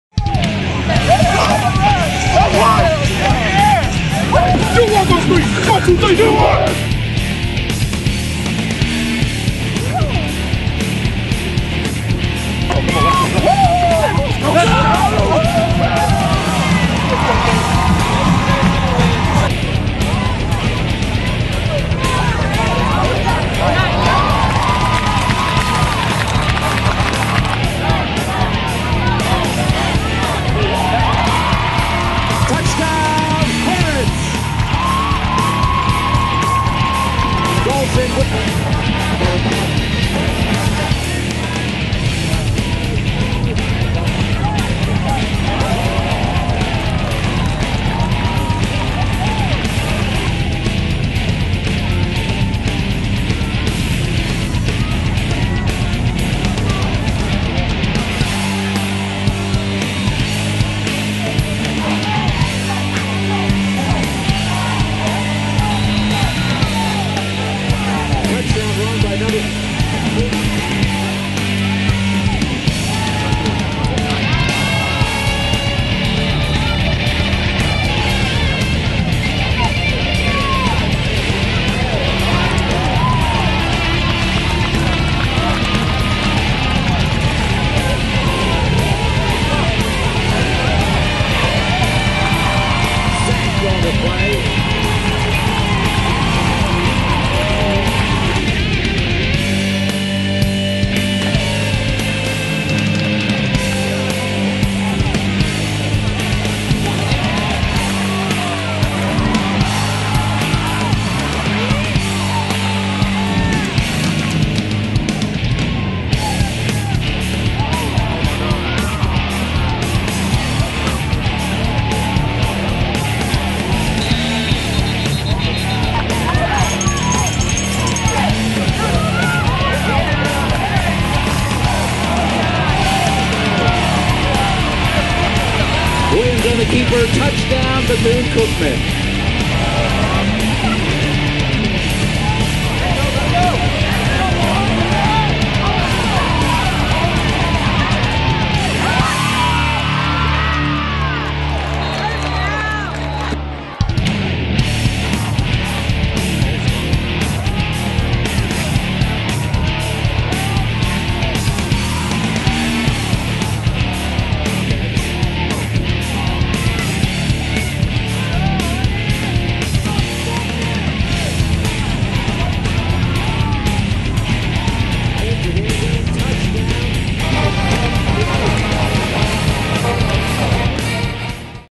Football Highlights: Delaware State Hornets vs. No. 21 Bethune-Cookman Wildcats
The Hornets fell 21-7 to the No. 21 Bethune-Cookman Wildcats in their
home opener on Saturday, Oct. 5.